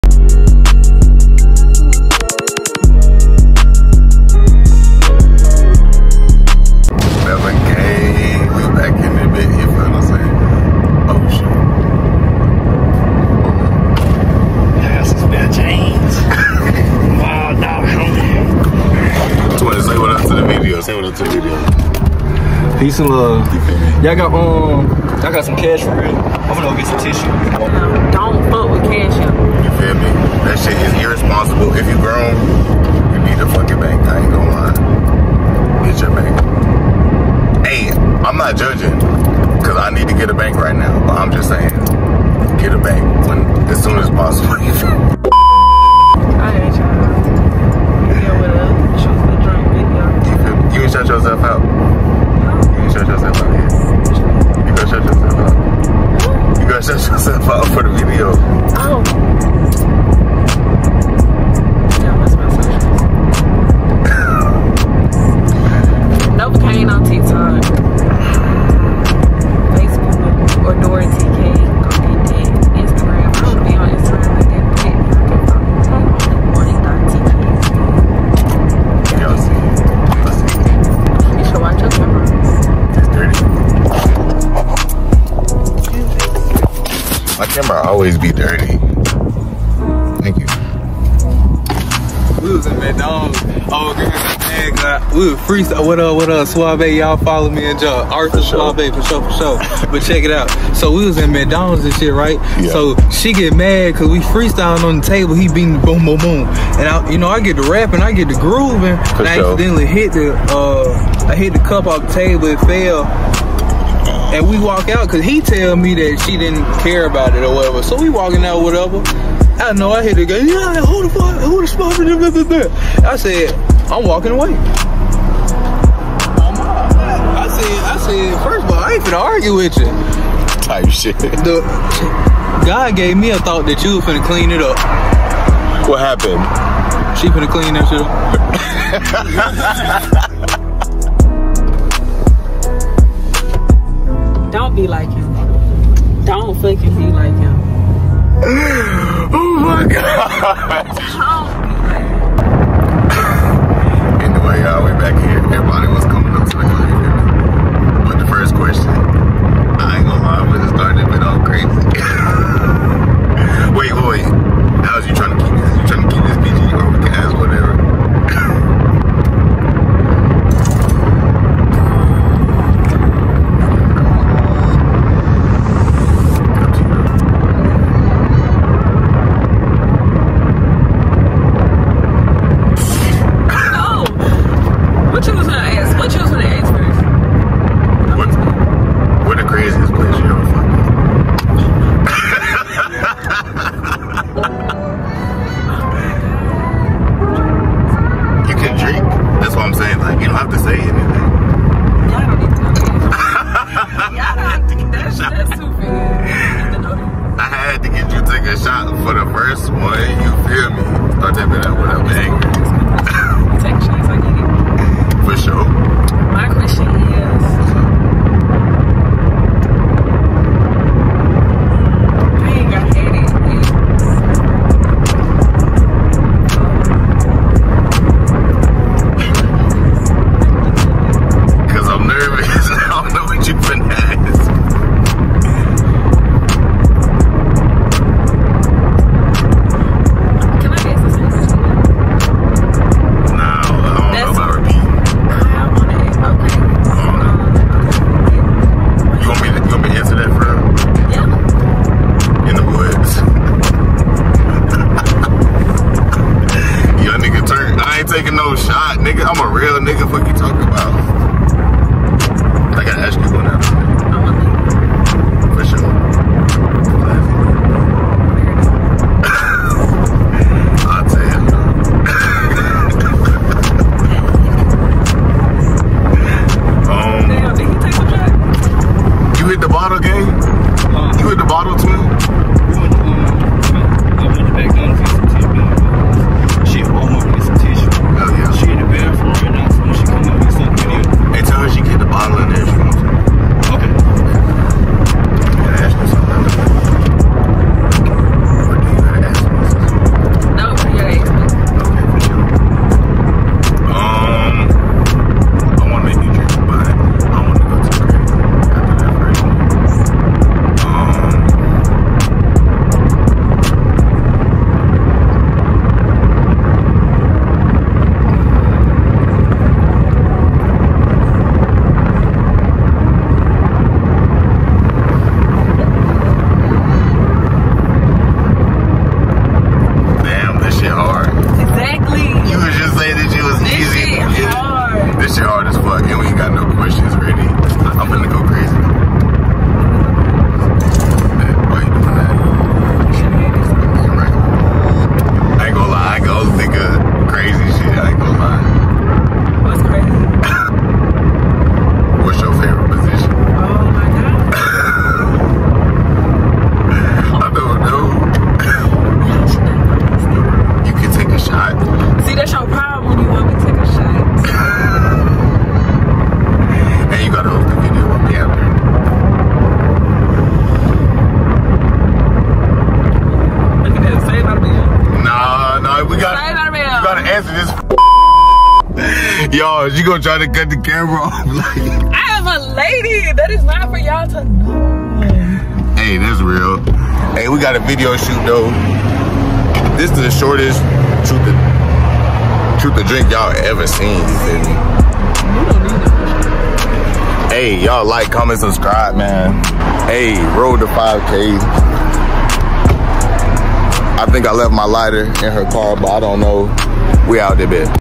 Truth or Drink Ft Onlyy1.tk
Another banger for y’all !! A truth or drink ! I hope y’all enjoy !! I love y’all and appreciate all the support !!